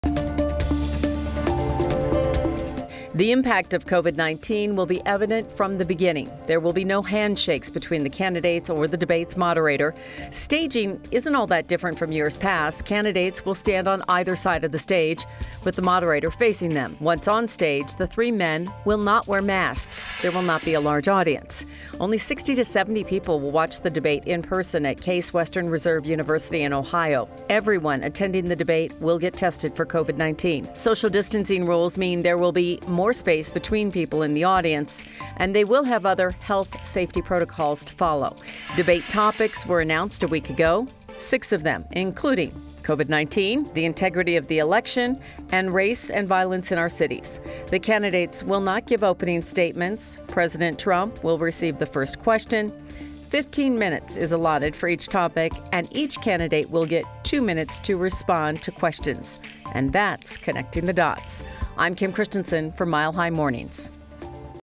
What to expect in first Biden-Trump presidential debate
President Donald Trump and Democrat Joe Biden will meet in their first debate Tuesday. Here's what to expect.

9NEWS (KUSA) is located in Denver, Colorado.